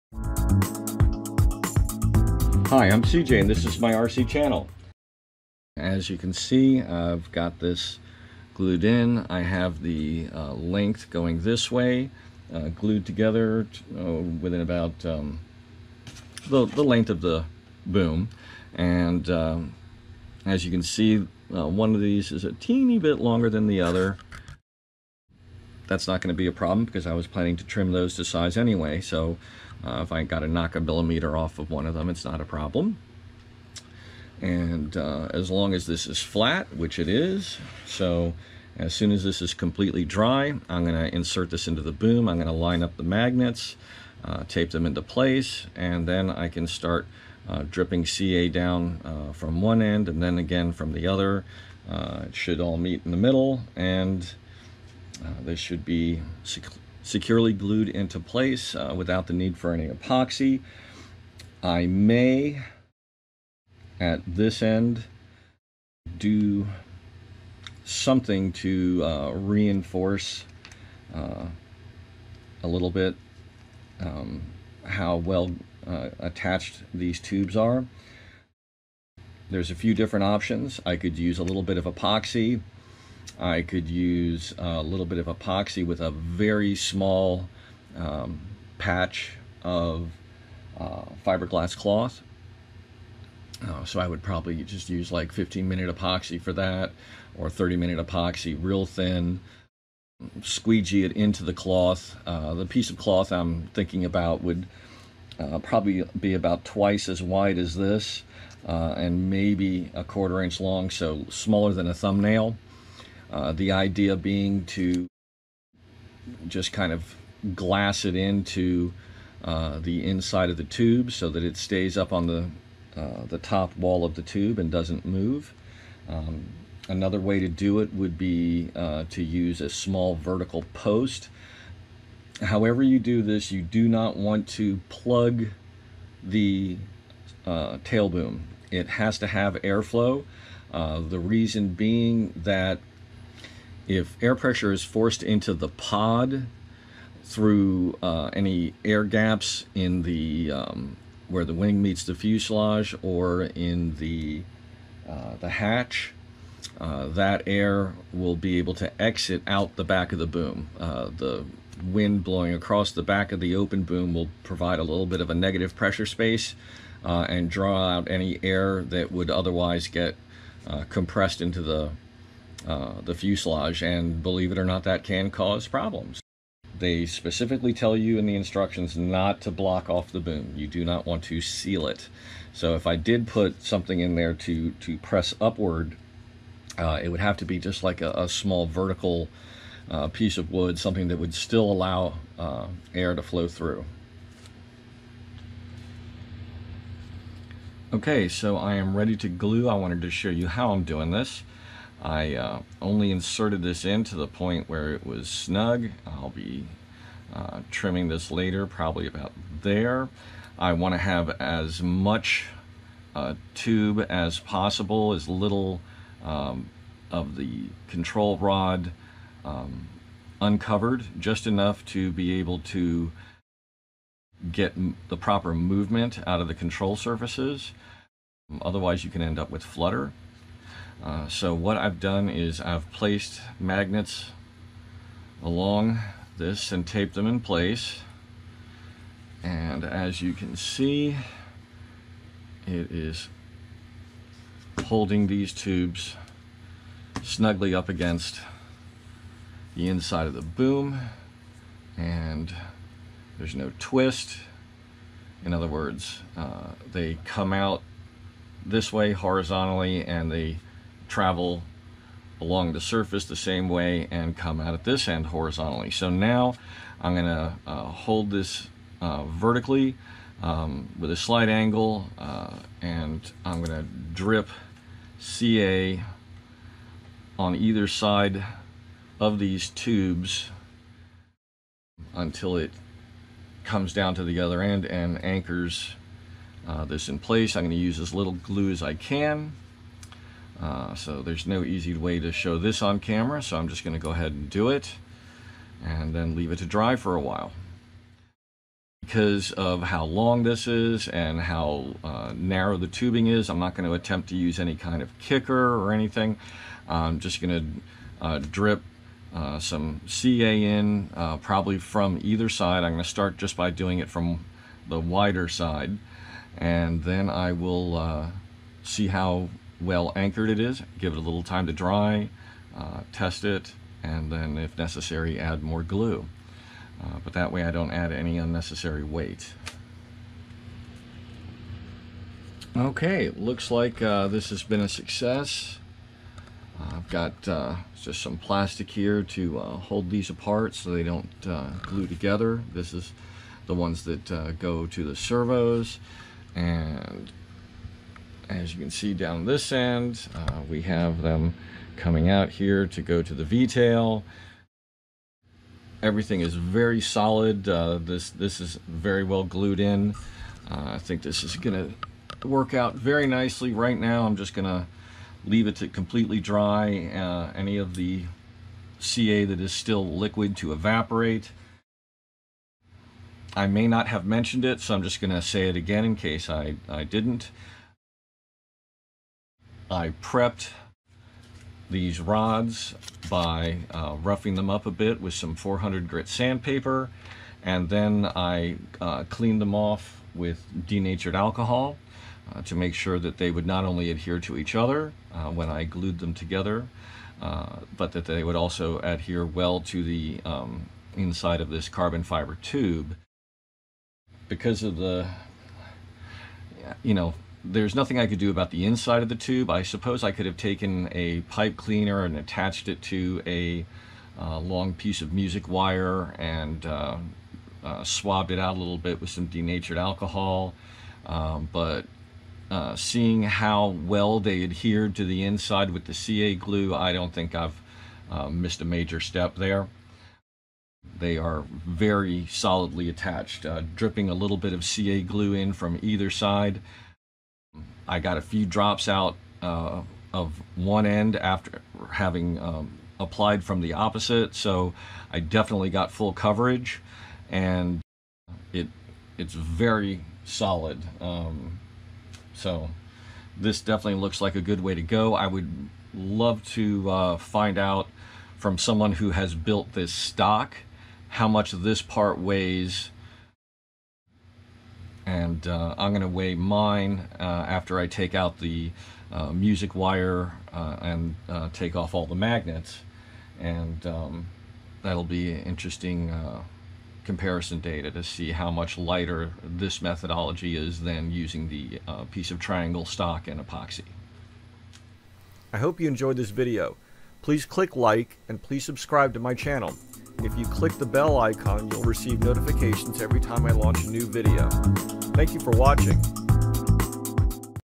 Chrysalis-Lite RES Build - Pt3b - Gluing the Pushrod Housings into the Tail Boom
In this video I complete glueing the pushrod housings into the tail boom using music wire and magnets. The Chrysalis-Lite F3-RES 2 meter sailplane designed for the RES or Rudder, Elevator, Spoiler spec. I bought it to compete in 2 meter TD or Thermal Duration events. The kit is optimized for lightness and strength while retaining the great handling characteristics of DJ Aerotech’s family of gliders. It costs $180.00 and is sold directly through DJ Aerothech’s website (link below). The RES competition spec is designed to keep the gliders in this class inexpensive and attractive to both beginners and experienced pilots. The kits must be mostly wood construction and controls are limited to rudder, elevator, and spoilers. This means that the planes can be flown without the need for an advanced programmable radio. The RES class is very popular in Europe and hopefully, it will catch on in the US as well.